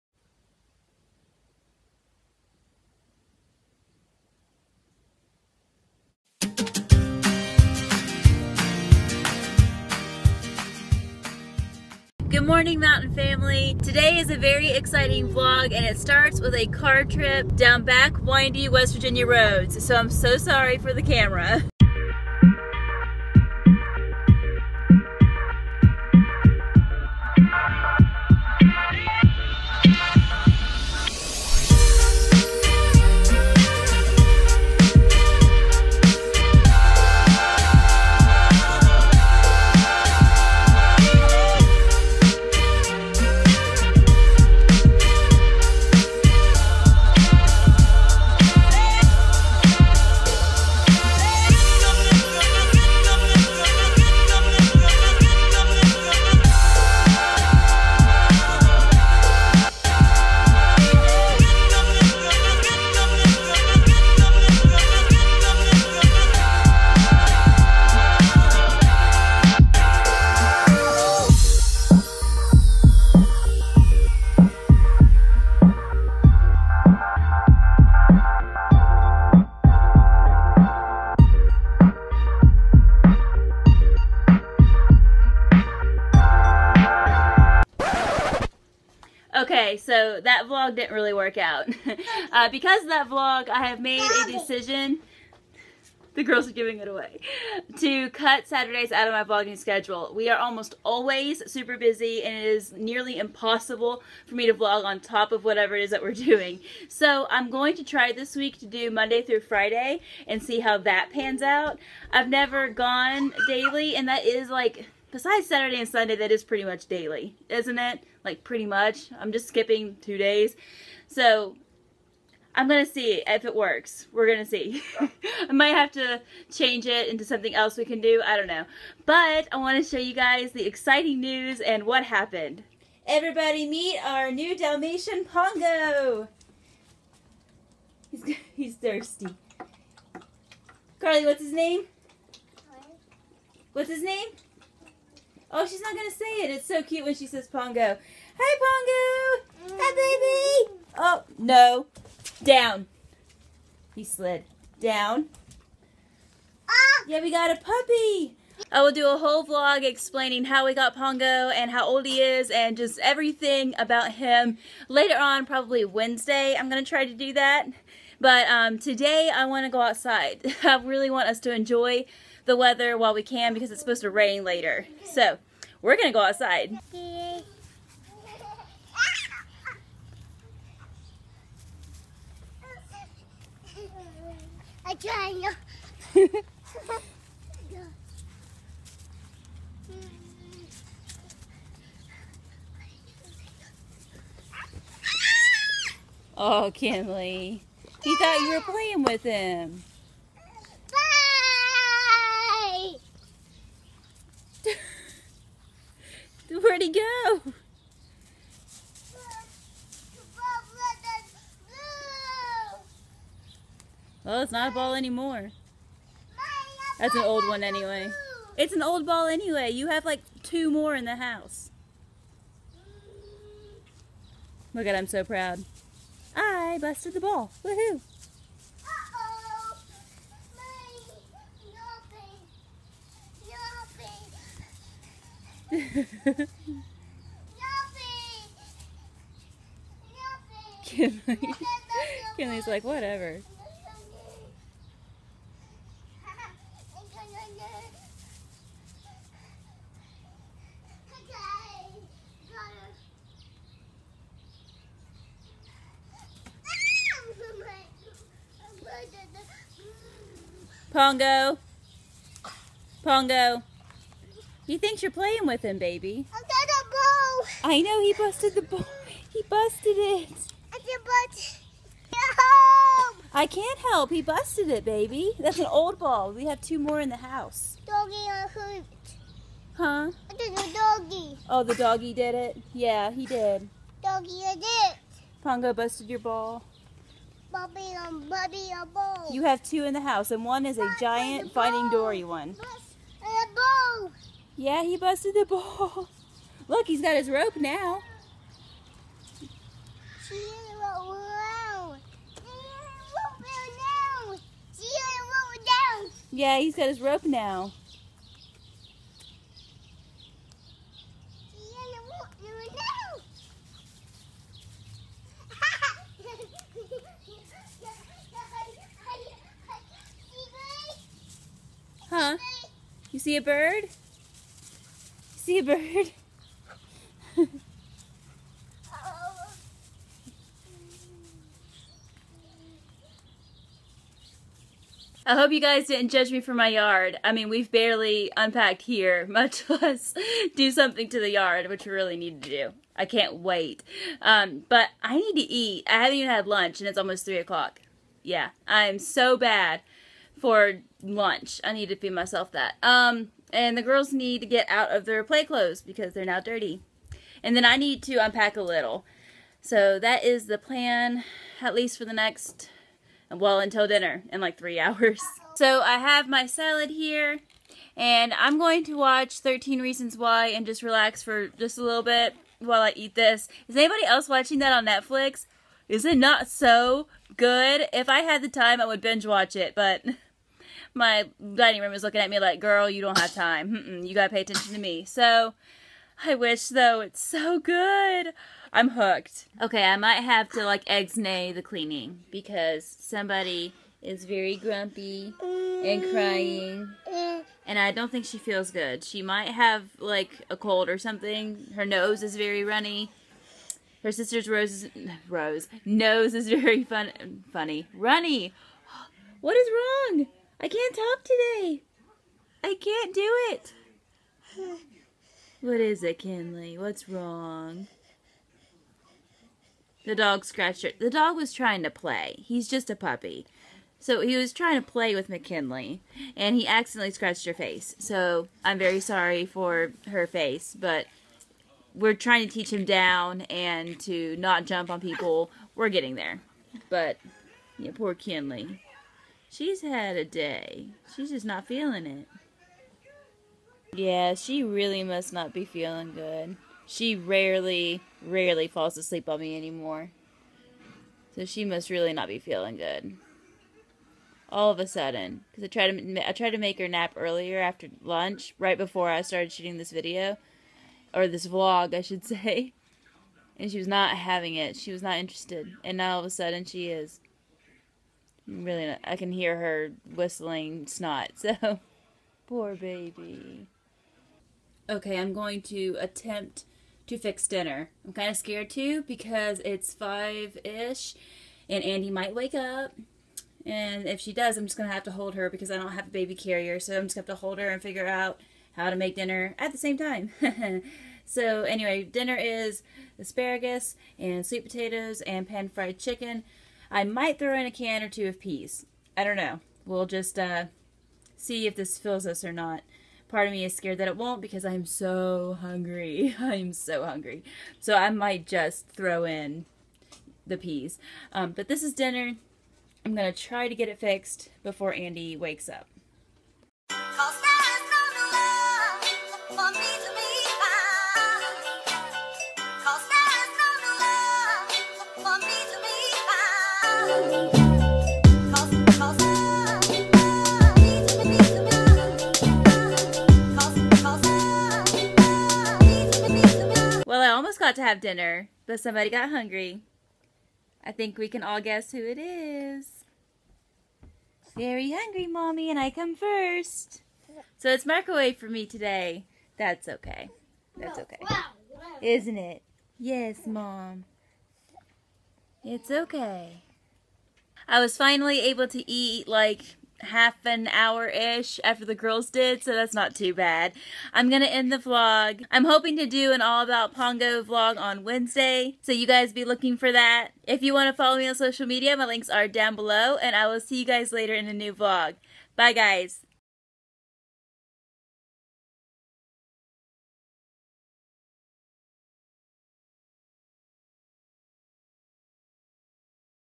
NEW FAMILY MEMBER!
~~OPEN ME~~

Hey Mountain Family! So over the weekend we got a new family member and we're so in love! Wednesday I will dedicate a whole vlog about him and tell you guys everything. Hope you guys enjoy the new vlogging schedule! Hopefully it works out better :) see you soon fam!

~~SOCIAL MEDIA~~